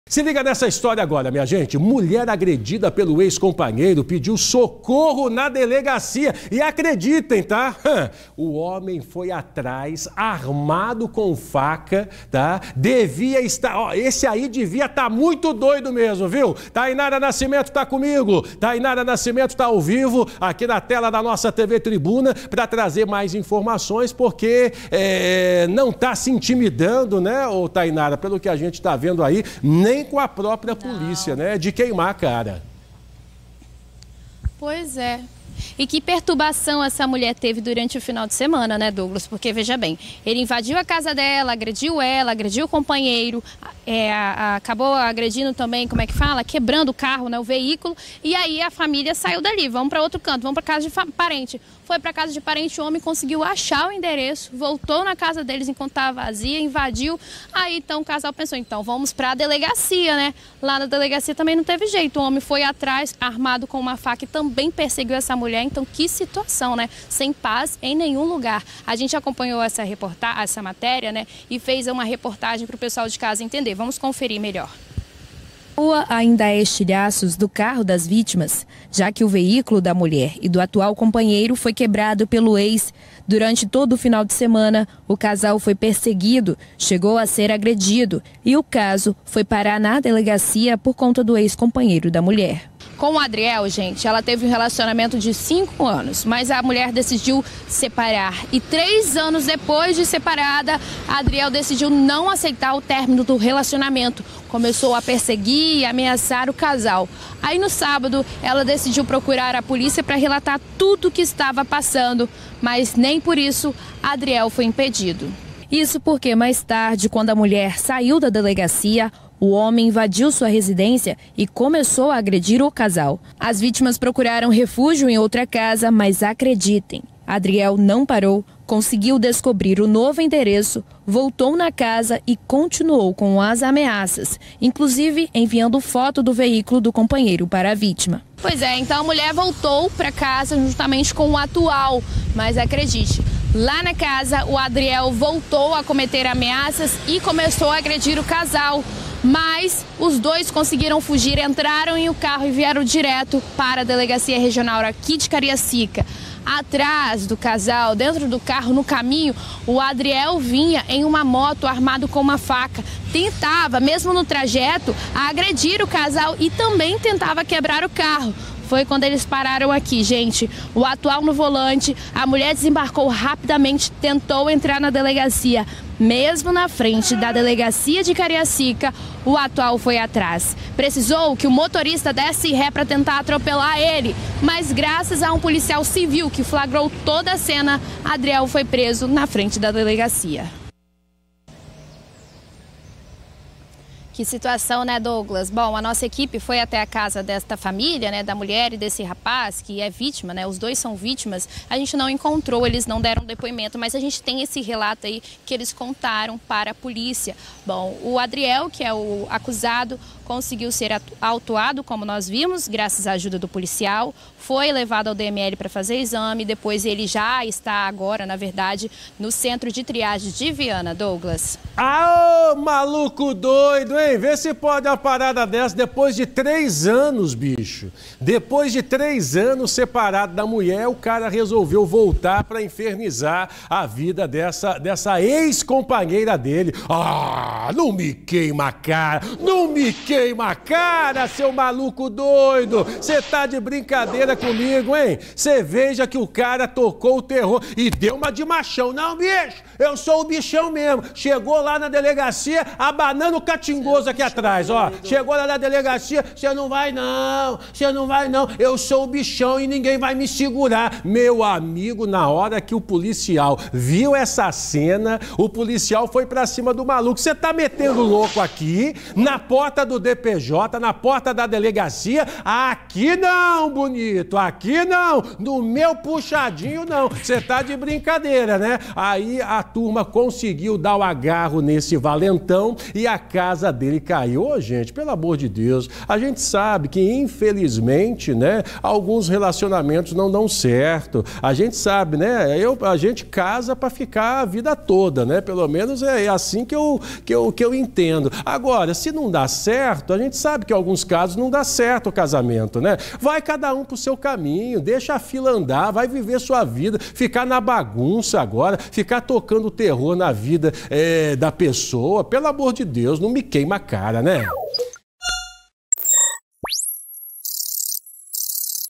Mulher é ameaçada por ex-companheiro armado na porta de delegacia
Vítima conseguiu entrar correndo na delegacia e pedir ajuda de um policial civil.